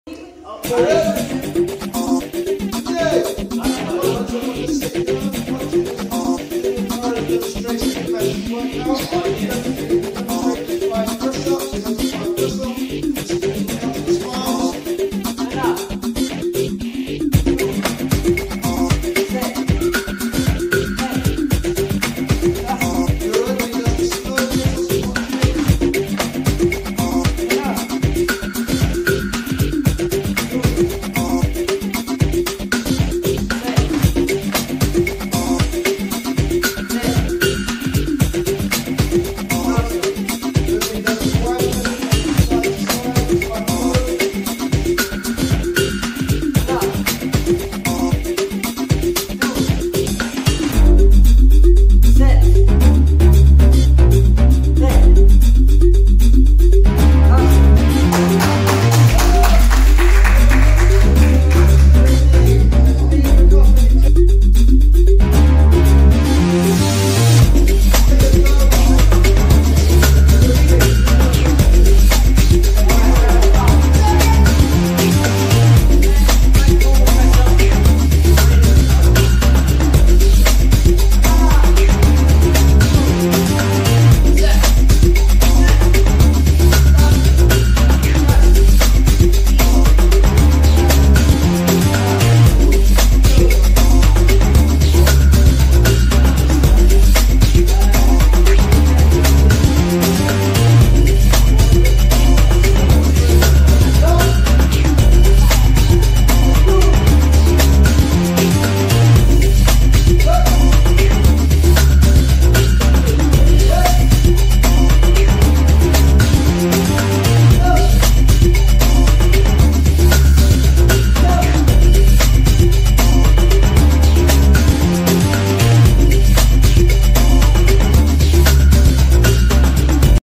Dylan Vekaria LEADERSHIP TAEKWONDO HIIT November 2019
Senior student Mr Dylan Vekaria of Friern Barnet Taekwon-Do School leading the students through our in house Daniel HIIT MAN Chiu chest workout 🏅👍🏾🏅.
Well done and keep on rising up in excellence great Leadership Dylan 👊🏾💫👊🏾 your Instructor Mr Malik Ayman.